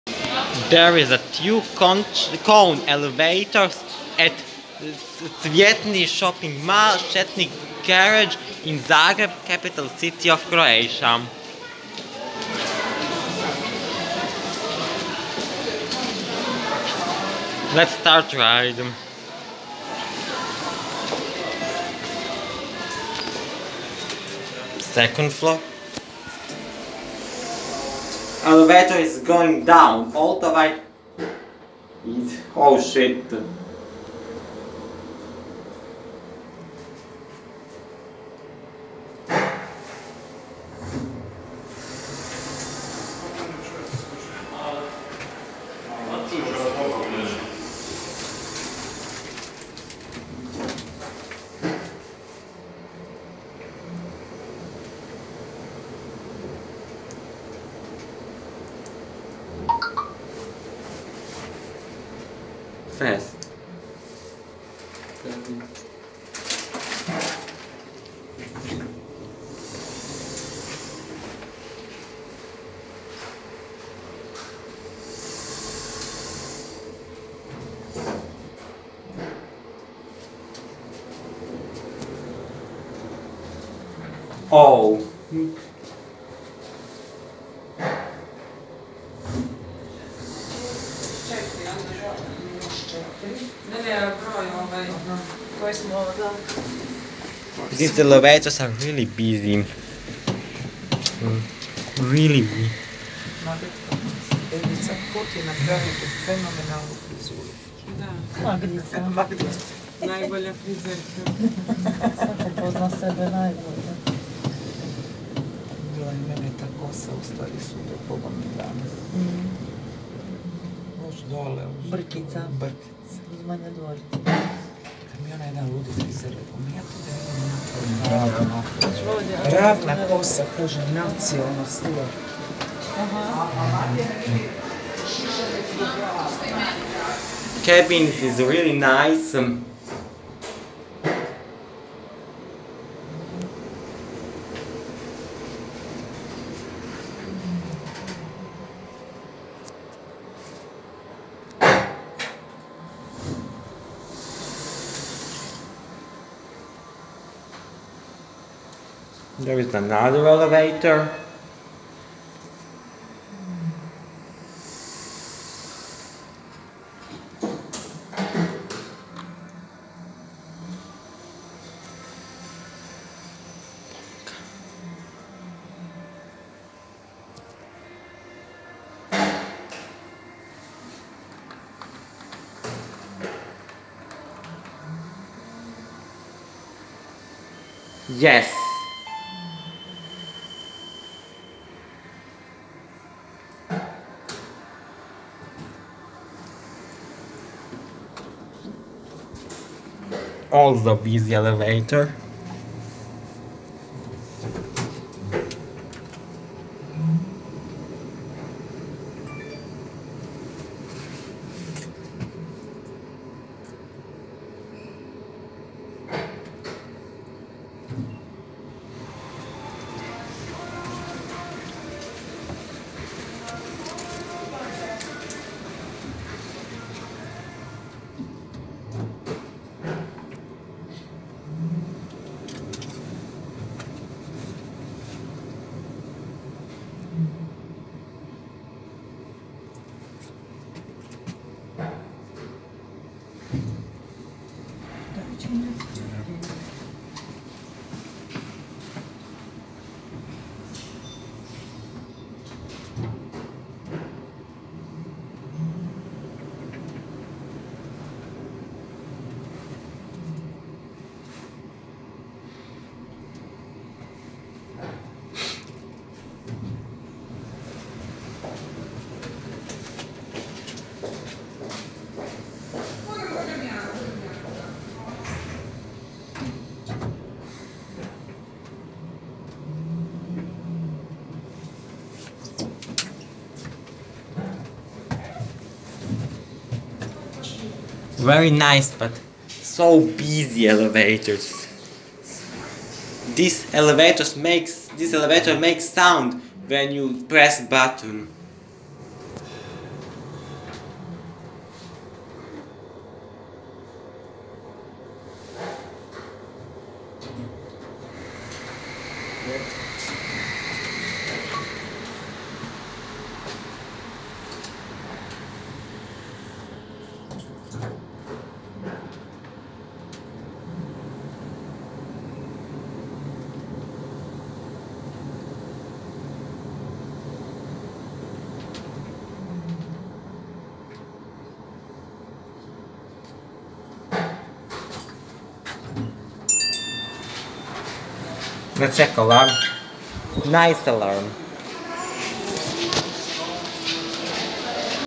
BUSY: Two 2011 Kone elevators at Cvjetni shopping centre and parking garage, Zagreb, Croatia
In this video you can see two nice Kone elevators at Cvjetni parking garage  in Zagreb, Croatia.
Also these elevators are very busy, as you can see in video.

Brand: Kone
Location: Cvjetni shopping centre and parking garage, Trg Petra Preradovića 6, Zagreb, Croatia.
Capacity: 1010kg or 13 persons
Year: 2011